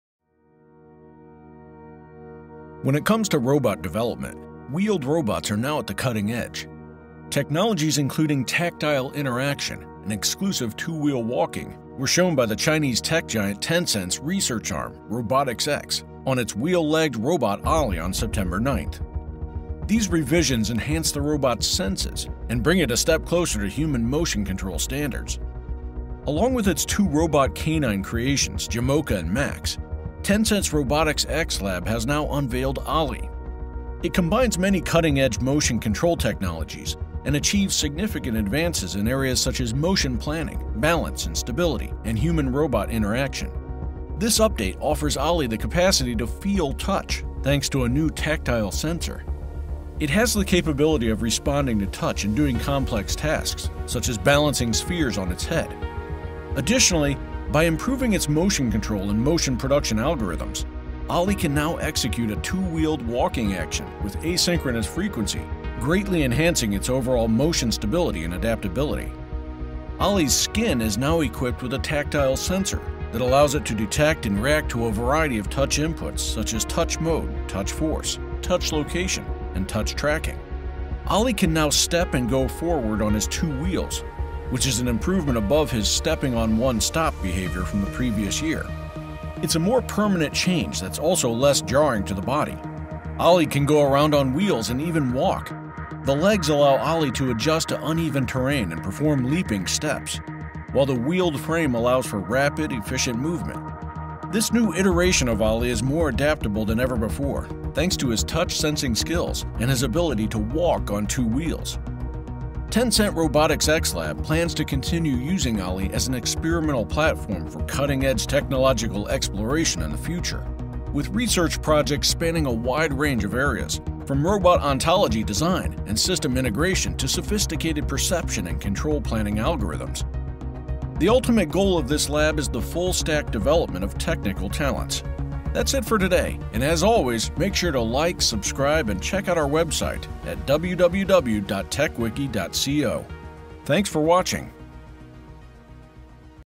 Tencent Reveals Progress of Wheel-Legged Robot Ollie
Ollie is "a revolutionary wheel-legged robot" created by Tencent Robotics. Ollie has various new features, including planar parallel legs resting on powered wheels. In addition to its outstanding acrobatic abilities, the robot can change its form from a biped to a triped so that it may stand very erect and hold a coffee-carrying manipulator.